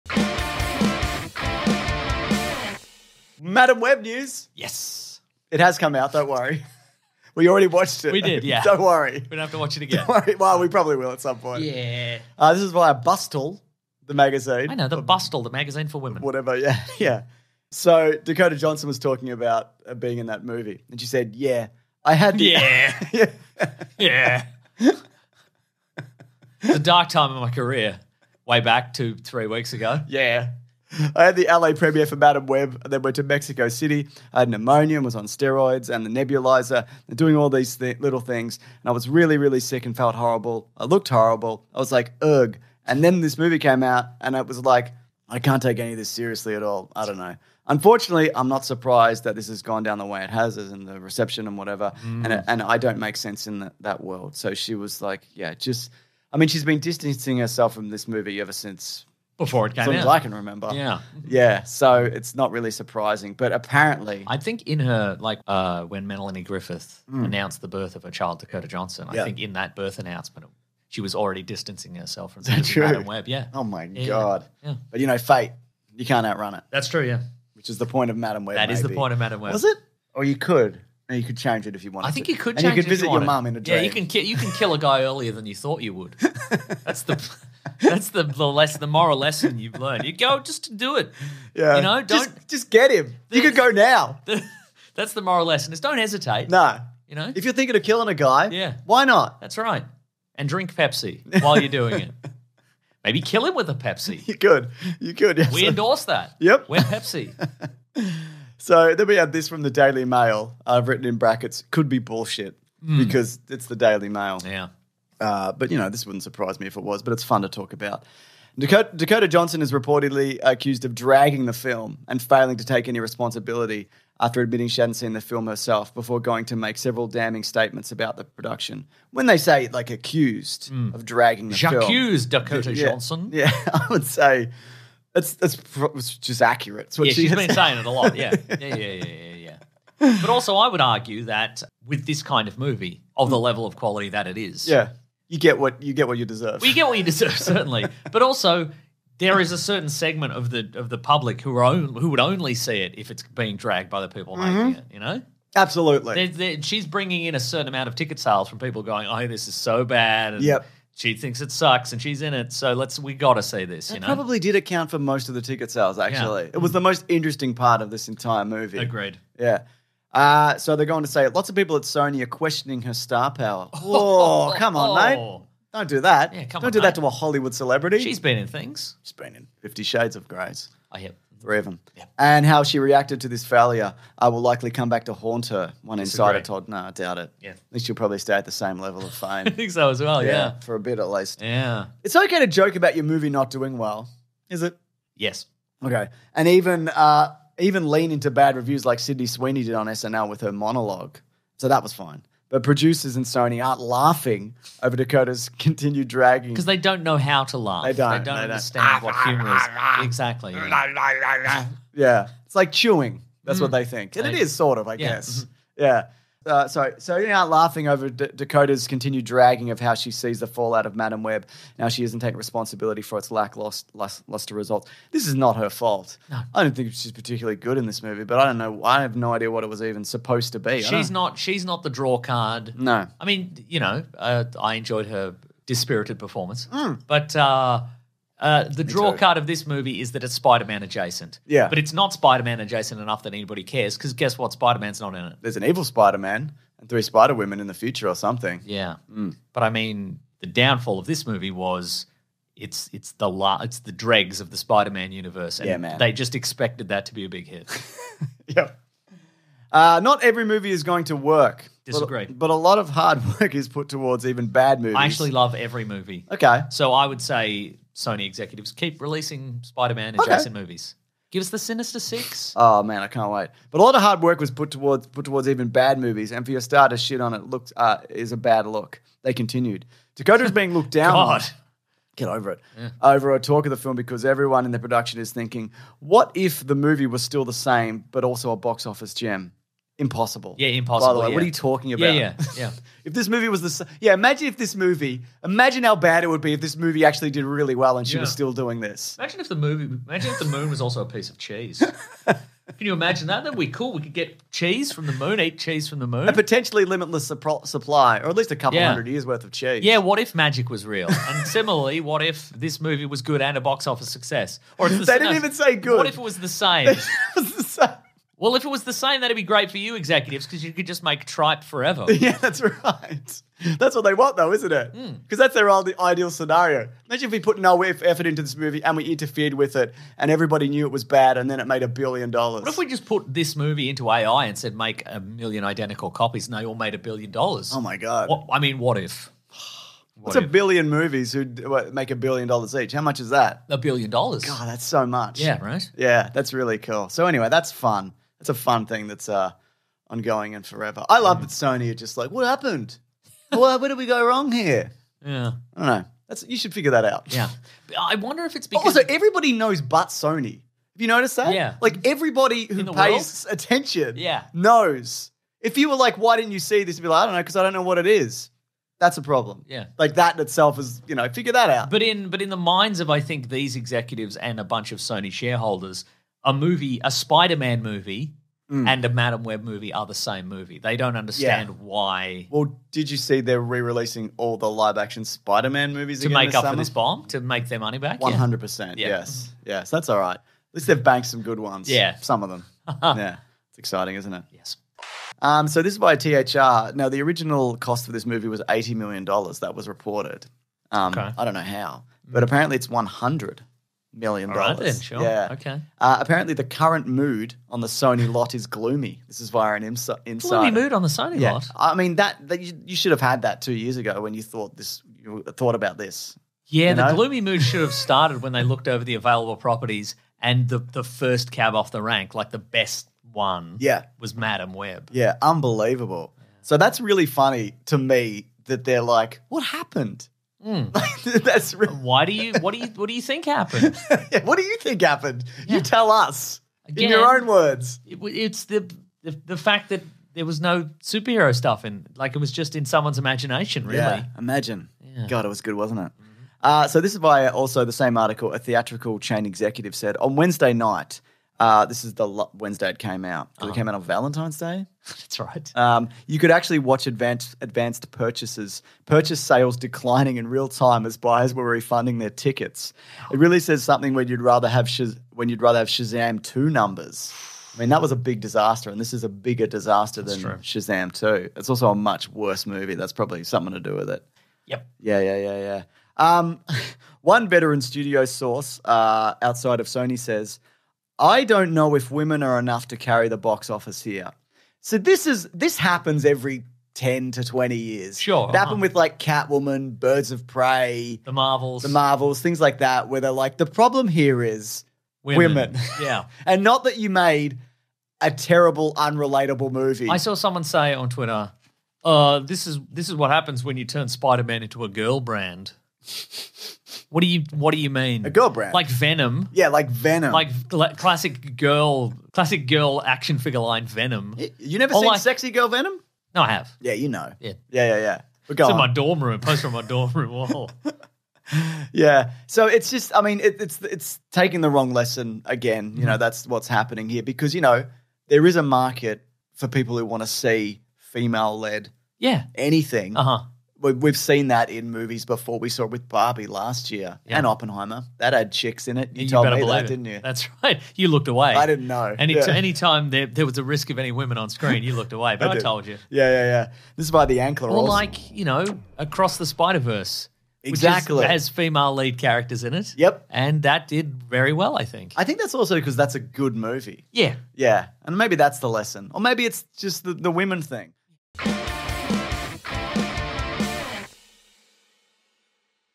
Dakota Johnson vs Madame Web: The Saga Continues
Dakota Johnson was recently interviewed by Bustle and she gave some interesting perspective on her Madame Web experience. We also discuss Sony's reaction to Dakota's comments about the film and their surprise at the movie not doing well. What could have possibly gone wrong? Everything.

Visit bigsandwich.co for a bonus weekly show, exclusive movie commentaries, early stuff and ad-free podcast feeds for $9 per month.